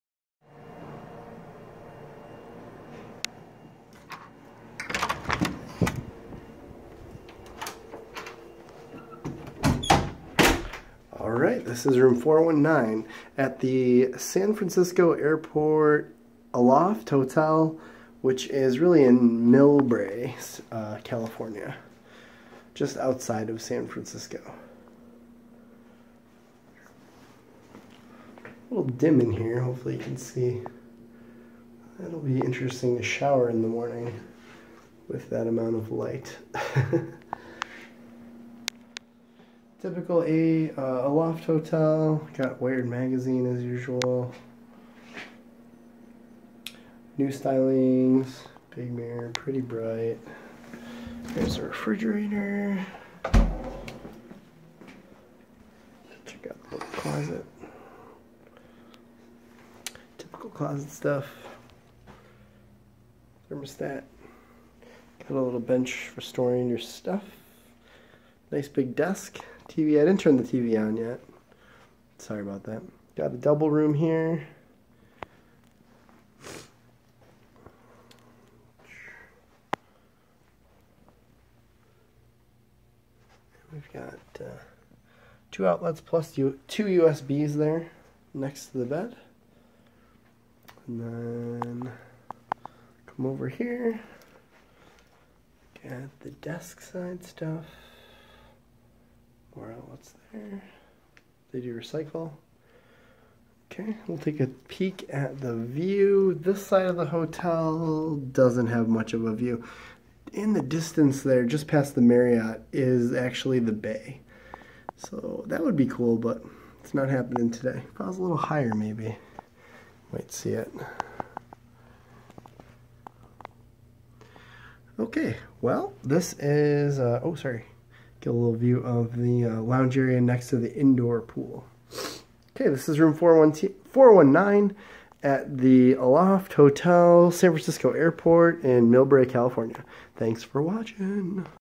Aloft Hotel Room Review San Francisco Airport
Review of room 419 at the Aloft Hotel near San Francisco Airport in Milbrae, CA.